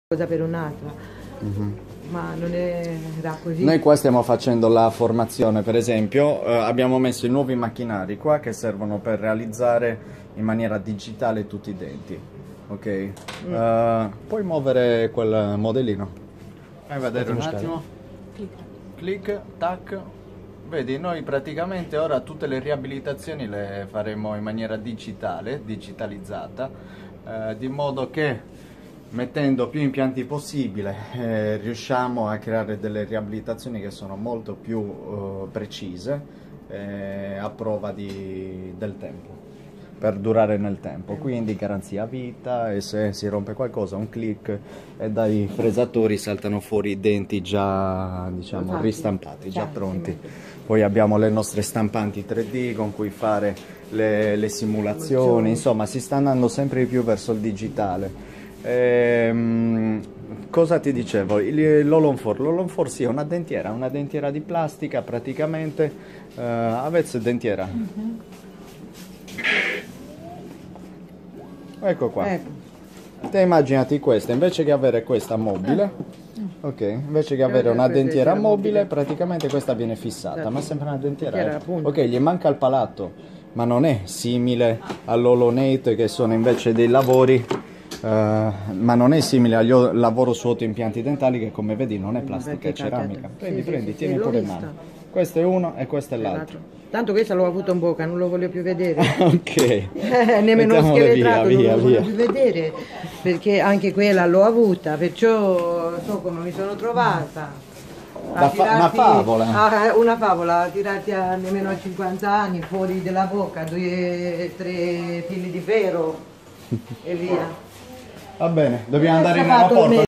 Dr Palmas : Odontoiatria Digitale per denti fissi su impainti DENTALI
In questo video spiego come sarà il futuro delle protesi dentali fisse,

utilizzo di nuovi macchinari e e materiali.
Se oggi i denti fissi in zirconio sono il massimo non sarà così in futuro.

Nel prossimo futuro i denti fissi su impianti saranno fatti con materiali rivoluzionari che magari ancora non esistono.